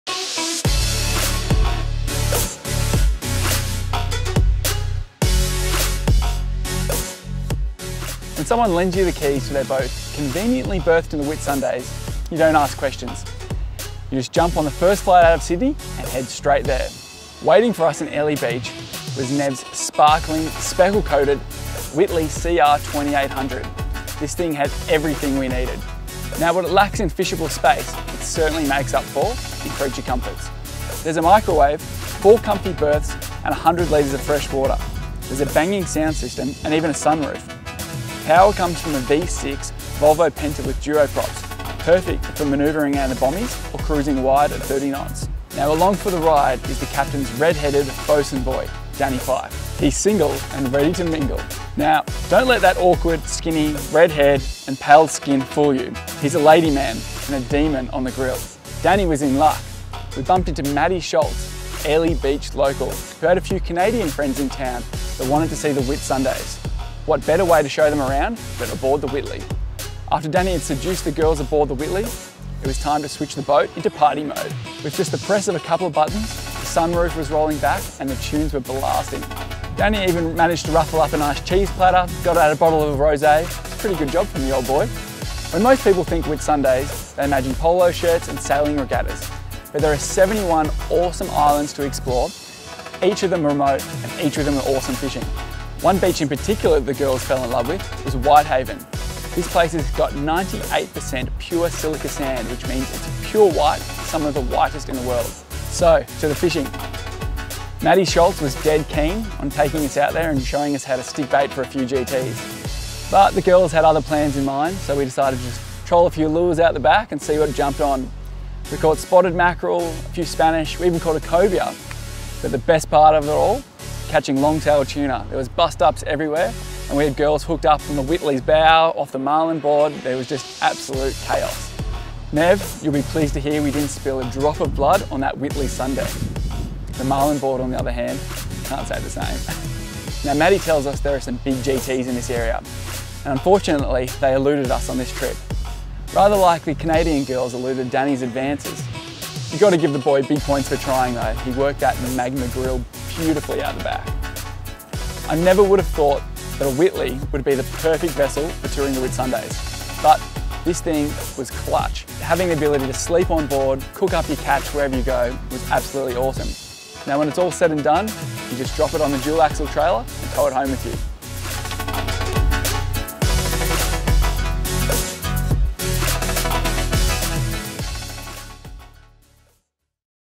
Whittley CR 2800 Whitsundays Adventures
Come join the Captain Magazine as they explore the Whitsnday Islands in the luxury of the Whittley CR 2800.